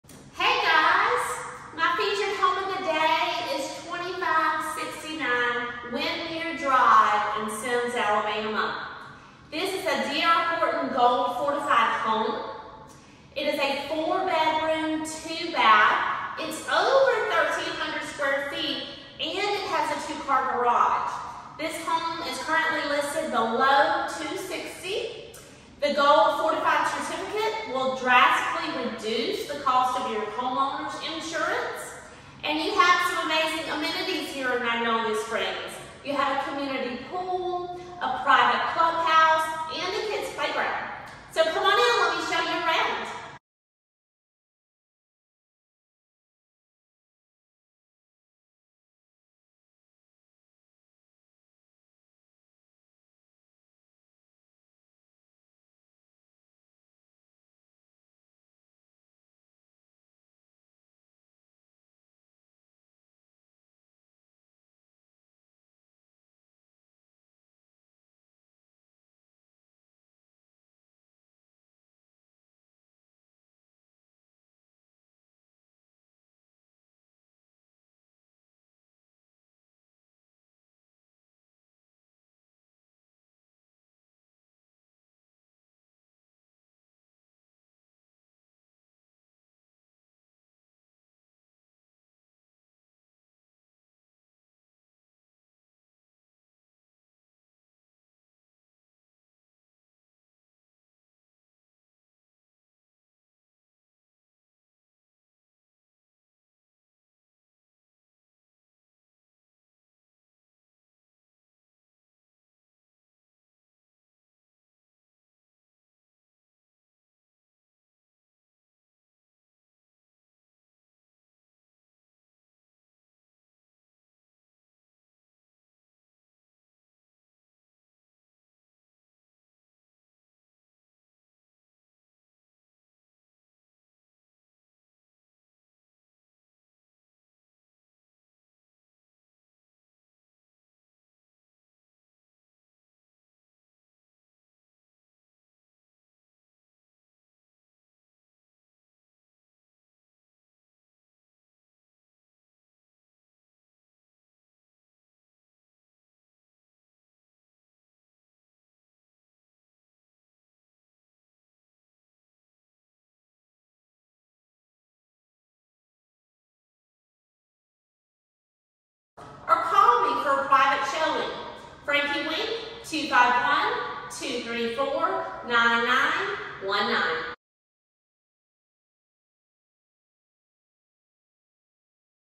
2569 Windmere Dr E Semmes Alabama Intro And Full Tour
2569 Windmere Dr  E Semmes Alabama 36575

Presenting the alluring FREEPORT floor plan, boasting 4 bedrooms and 2 baths in an open layout that's perfect for hosting guests. The kitchen and bathrooms showcase stylish cabinets and stainless steel appliances to meet your culinary needs, including a spacious island. The home features elegant vinyl plank flooring and plush bedroom carpeting. Relax outdoors on the covered front and back porches. Plus, enjoy the convenience of a two-car garage.

Introducing the cutting-edge NEW SMART HOME SYSTEM! This includes a Kwikset smart code front door lock, Sky-Bell video doorbell, Eaton Z-wave light switch for remote control, and the Honeywell T6 Pro Z-Wave thermostat for seamless smartphone or tablet integration. Communicate with your smart home using the Amazon Echo Dot voice-command device, giving you full control over lights and door locks. This home is Gold Fortified Certified, potentially reducing homeowner's insurance costs, and comes with a 1-year builder's warranty and a 10-year structural warranty. The seller's offer stands to cover up to $4000 of the buyer's closing costs when working with the preferred lender, DHI Mortgage.

Frankie Wink - Realtor
Legendary Realty